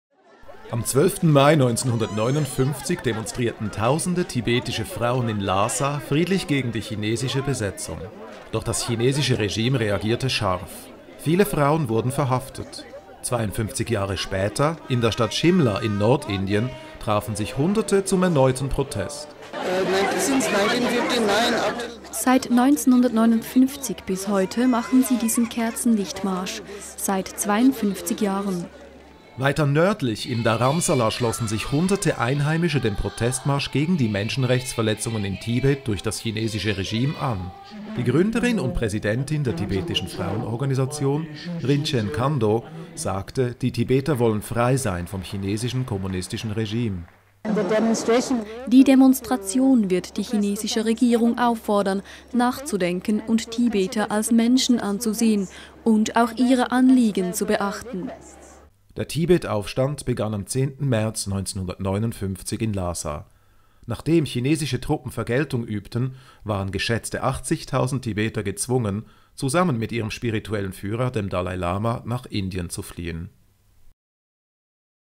Hunderte Tibeter protestieren in Indien gegen das Chinesische Regime
Am 12. Mai 1959 demonstrierten Tausende tibetische Frauen in Lhasa friedlich gegen die chinesische Besetzung.

Doch das chinesische Regime reagierte scharf, viele Frauen wurden verhaftet. 52 Jahre später, in der Stadt Shimla in Nord-Indien, trafen sich Hunderte zum erneuten Protest.

[Frau Passang, Aktivistin, Tibetan Women's Association]:
„Seit 1959 bis heute machen sie diesen Kerzenlichtmarsch, seit 52 Jahren."

Weiter nördlich in Dharamsala schlossen sich hunderte Einheimische dem Protestmarsch gegen die Menschenrechtsverletzungen in Tibet durch das chinesische Regime an.

Die Gründerin und Präsidentin der tibetischen Frauenorganisation, Rinchen Khando, sagte, die Tibeter wollen frei sein vom chinesischen kommunistischen Regime.

[Rinchen Khando, Präsidentin, Tibetan Women's Association]:
„Die Demonstration wird die chinesische Regierung auffordern, nachzudenken und Tibeter als Menschen anzusehen und auch ihre Anliegen zu beachten."

Der Tibet-Aufstand begann am 10. März 1959 in Lhasa. Nachdem chinesische Truppen Vergeltung übten, waren geschätzte 80.000 Tibeter gezwungen, zusammen mit ihrem spirituellen Führer, dem Dalai Lama, nach Indien zu fliehen.